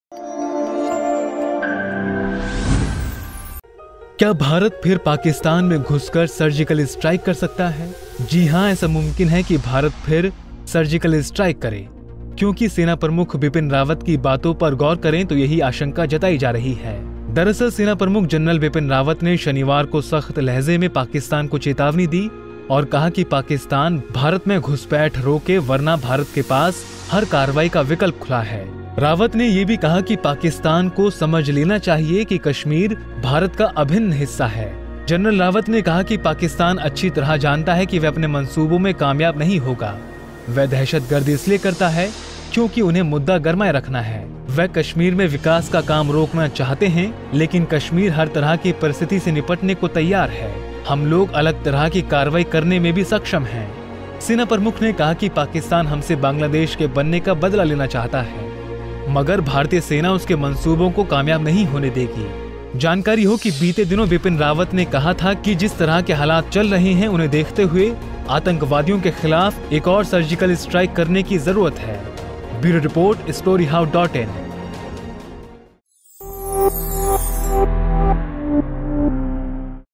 क्या Indian army फिर से surgical strike कर सकती है? जानिए क्या कहा सेना प्रमुख बिपिन रावत ने
bipin rawat | vipin rawat army | general bipin rawat |general of indian army |lieutenant general bipin rawat | bipin rawat